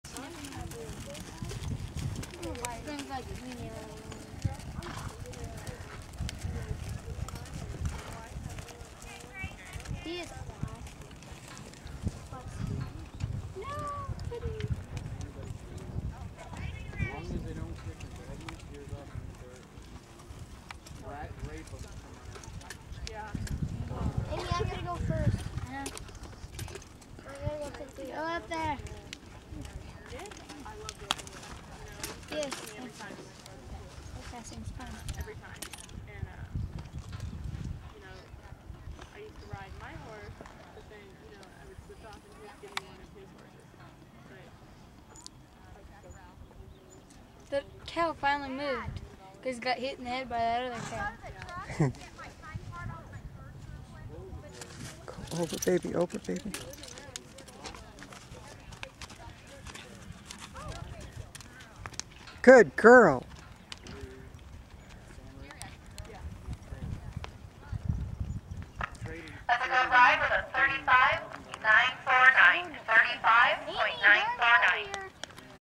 Dusty Spurs 4 17 2011 Keyhole 35.949
This was the last event, all we had to do was complete it with a "time" in order to win high point in FCA. We didn't take any chances and walked.